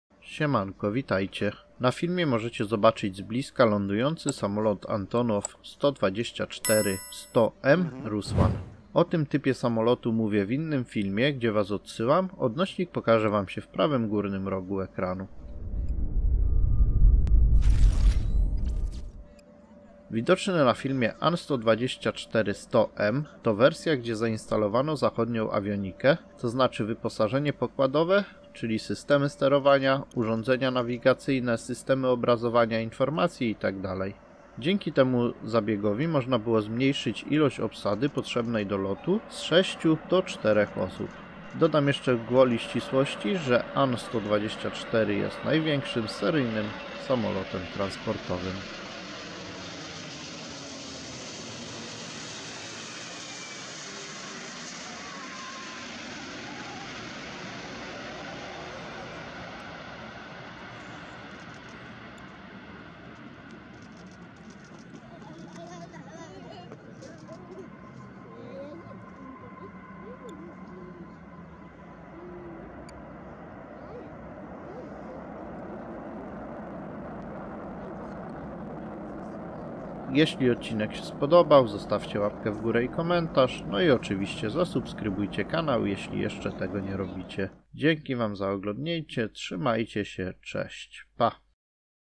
ANTONOV 124-100M Rusłan | Lądowanie z bliska na lotnisku EPRZ Rzeszów - Jasionka
►Zapraszam do obejrzenia playlisty SAMOLOTY, gdzie znajduje się więcej filmów z lądowań na lotnisku EPRZ Rzeszów - Jasionka

Siemanko, witajcie
Na filmie możecie zobaczyć z bliska lądujący samolot Antonov 124-100M Rusłan , o tym typie samolocie mówię w innym filmie gdzie was odsyłam, odnośnik pokaże sie Wam w prawym górnym rogu ekranu.
Widoczny na filmie An 124-100M to wersja gdzie zainstalowano zachodnią awionikę, tzn. wyposażenie pokładowe takie jak systemy sterowania, urządzenia nawigacyjne, system obrazowania informacji itd.
Dzięki temu można było zmiejszyć ilość obsady potrzebnej do lotu z sześciu do czterech osób.
Dodam jeszcze gwoli ścisłości że An-124 jest największym seryjnym samolotem transportowym.

Dzięki wam za ogladnięcie trzymajcie się, cześć, pa